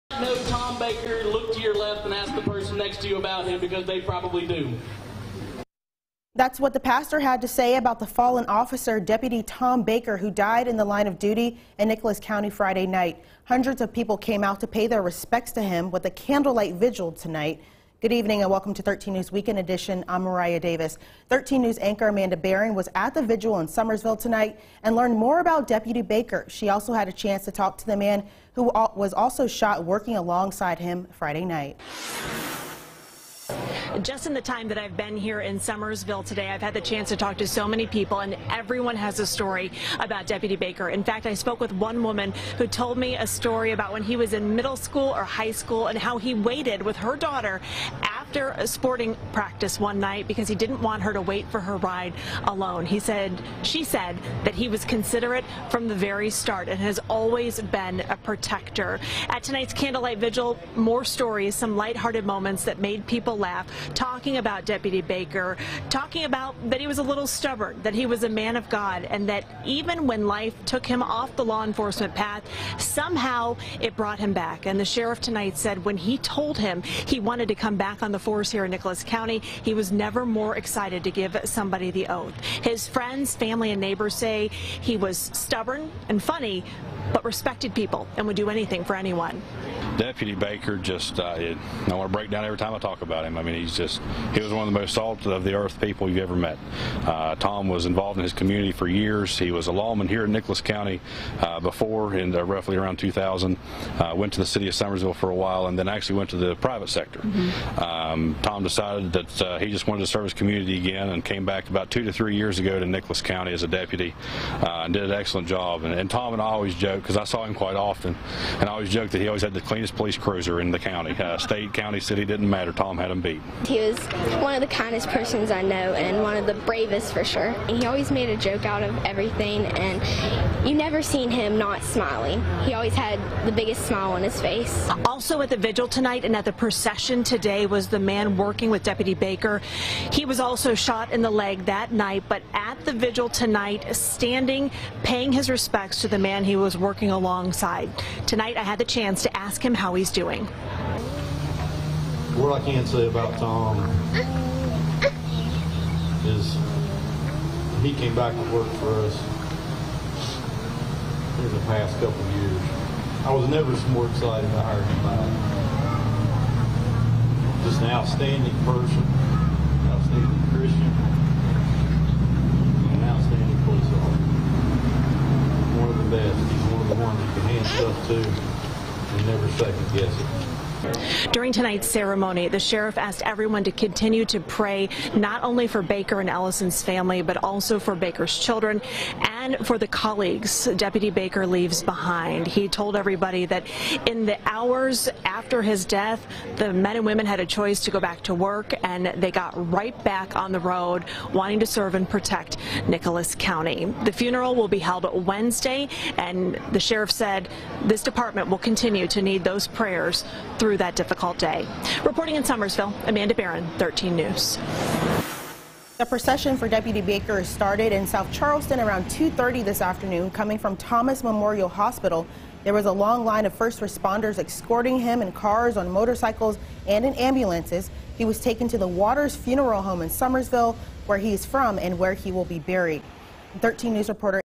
Community reflects and honors deputy killed in the line of duty
13 News Anchor Amanda Barren was in Nicholas County, where people from local and far away have already been paying their respects to honor a hero who died in the line of duty.